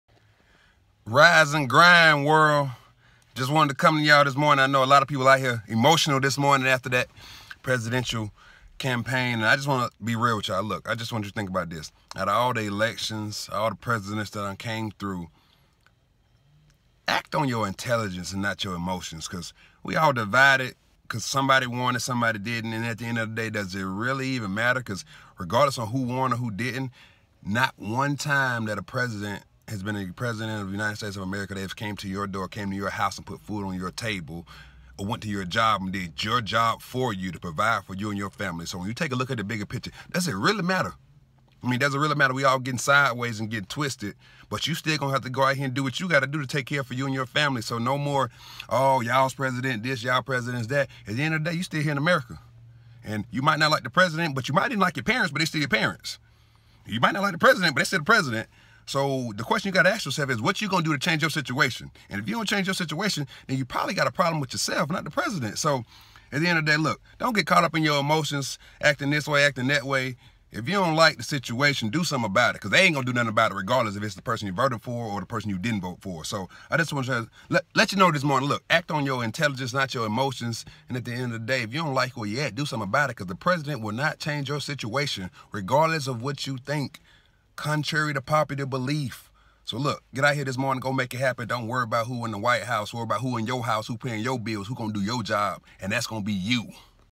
Presidential Election Won't change your situation
The President will NOT change your personal situation! So can we come together and act on our intelligence and not our emotions IIE Impact Inspire Empower derrickfurlow.com